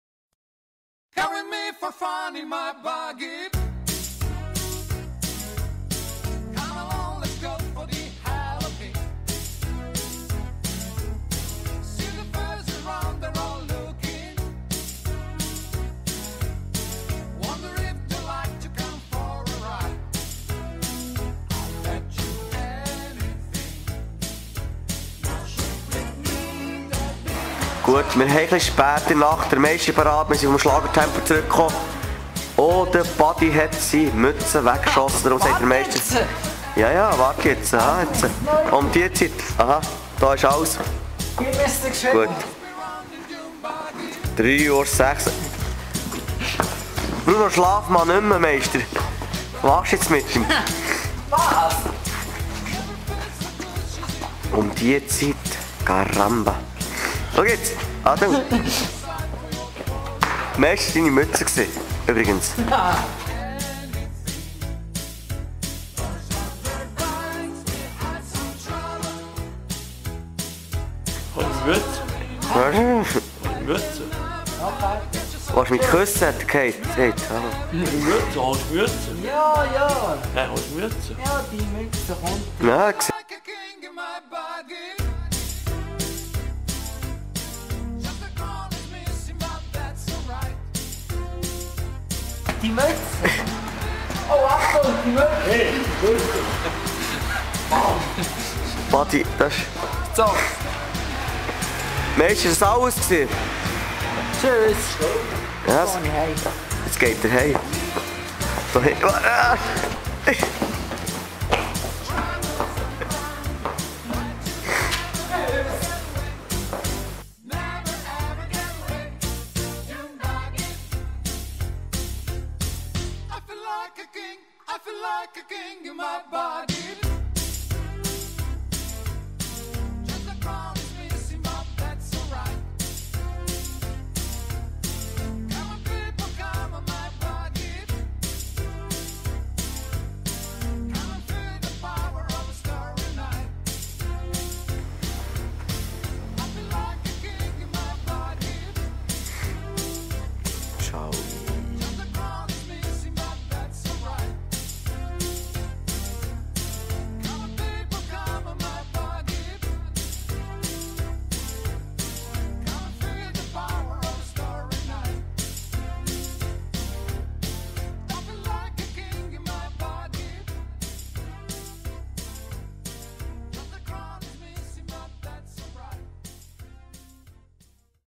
Buddy auf der Flucht
am Bahnhof Langenthal zu früher Stunde mit Buddy Love, Dodo & Troja